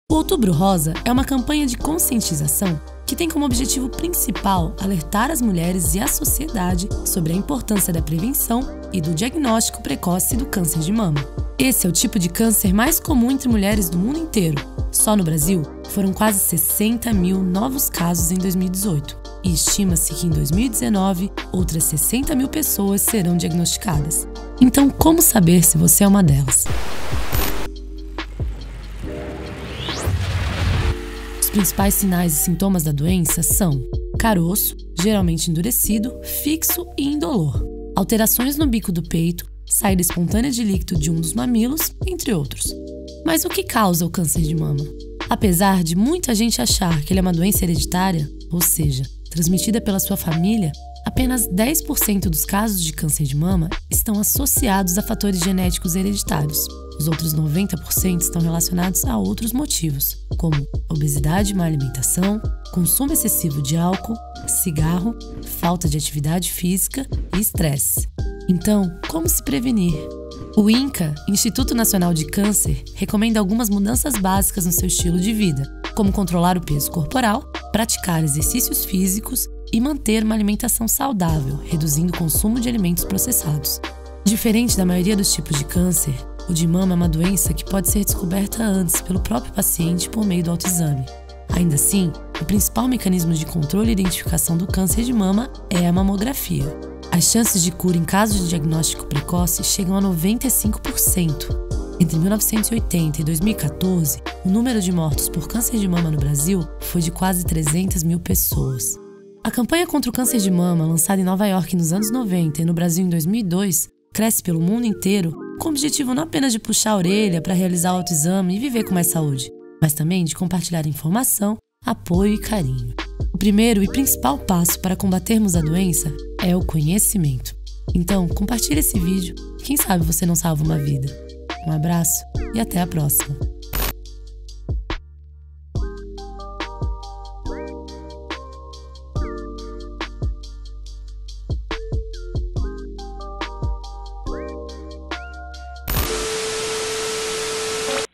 Outubro Rosa e o câncer de mama no Brasil
Ele é o câncer mais comum entre mulheres do mundo inteiro, e estimasse que em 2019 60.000 pessoas serão diagnosticadas com câncer de mama no Brasil. Como saber se você é uma delas? Quais os principais sintomas? E o que causa o câncer de mama? Será que um caso na família aumenta as suas chances? Hoje, o Laboratório 2000 vai solucionar essas e outras dúvidas comuns sobre o câncer de mama.

Créditos
Pesquisa, Roteiro e Narração: Leonardo Gonçalves de Souza
Edição e Imagens: Ricardo Gonçalves de Souza
Vídeo com caráter de uso transformativo para fins educativos.
Vídeo com legendas para deficientes auditivos.

Este canal faz parte do Science Vlogs Brasil, um selo de qualidade colaborativo que reúne divulgadores de